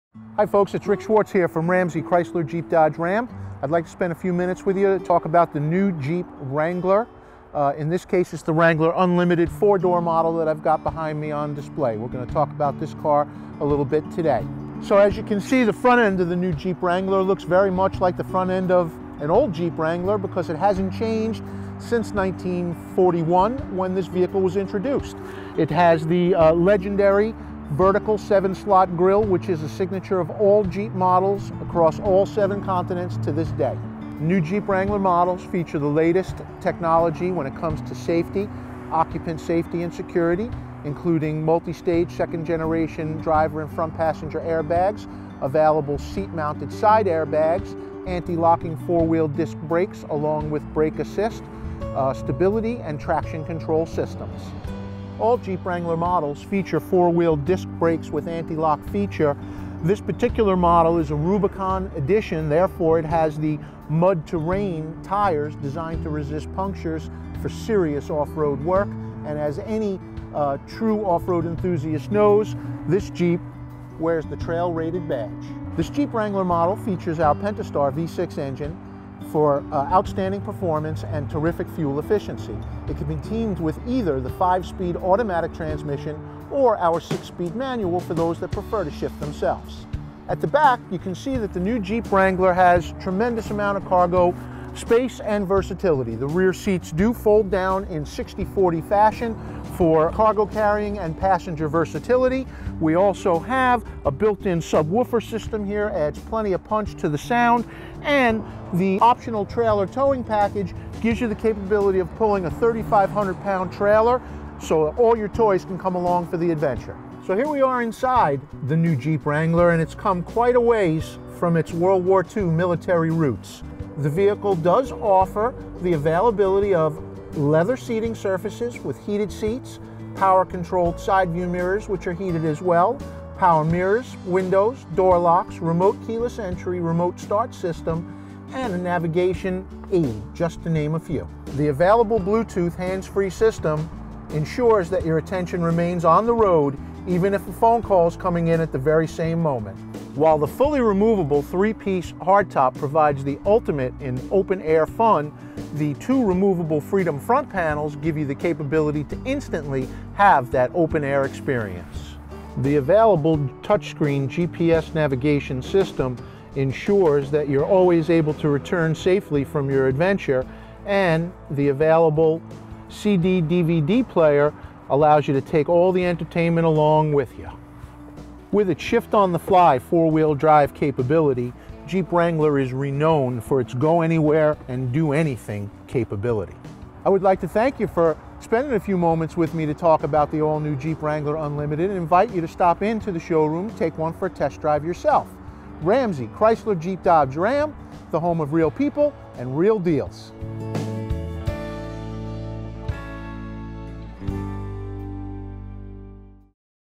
Tour the 2016 Jeep Wrangler Unlimited Rubicon 4x4 in NJ
Rick Schwartz at Ramsey Jeep gives a walkaround of the 2016 Jeep Wrangler Unlimited Rubicon at our dealership near Paramus, New Jersey. You’ll see this new SUV’s Trail Rated 4x4 badge and the Trailer Tow Package that lets you haul up to 3,500 lbs.

The Jeep Wrangler Unlimited has had the same styled front end since 1941 with features like the legendary 7-slot grille. This Rubicon model is also updated to meet the modern driver’s needs.

That’s because it equips convenience, comfort, safety and off-road features like:
2nd-generation multistage frontal airbags & available seat-mounted side airbags
4-wheel disc antilock brakes w/ brake assist
Stability & traction control systems
Mud-Terrain tires
Pentastar V6 w/ 5-speed automatic or 6-speed manual transmission
Leather seating surfaces
Heated front seats
60/40-split folding rear seats
Power heated side mirrors, windows & door locks
Remote keyless entry & start
Touchscreen navigation system
Bluetooth
CD/DVD player
Built-in subwoofers
Fully removable 3-piece hardtop & 2 removable Freedom front panels

Come test drive the 2016 Jeep Wrangler Unlimited Rubicon 4x4 at Ramsey Chrysler Jeep Dodge today! You can learn more about this versatile SUV by visiting